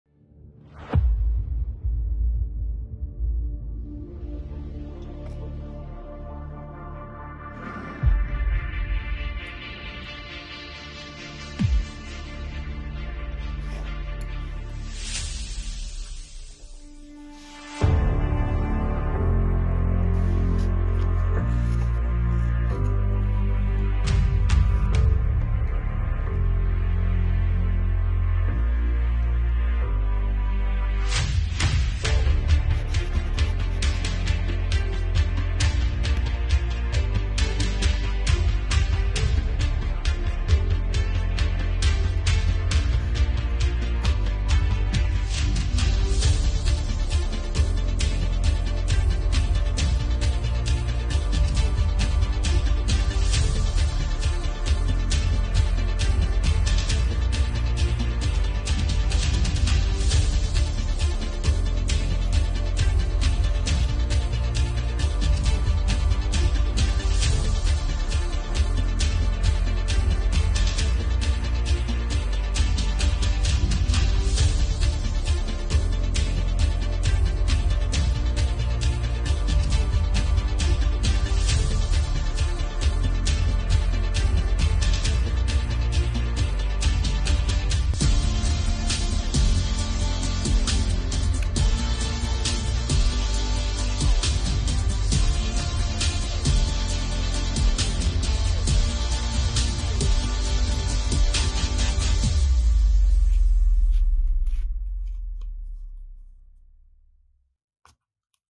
Making of - Handmade leather cardholder - Episode one (The pilot)
This 10-part series will include videos of the process of making handmade leather goods.

In this pilot episode we make a cardholder out of vegetable tanned leather called Pueblo. All leathergoods are made in Finland by a pair of hands.

Did you like it? Drop a comment and give us some advices to imporove our content.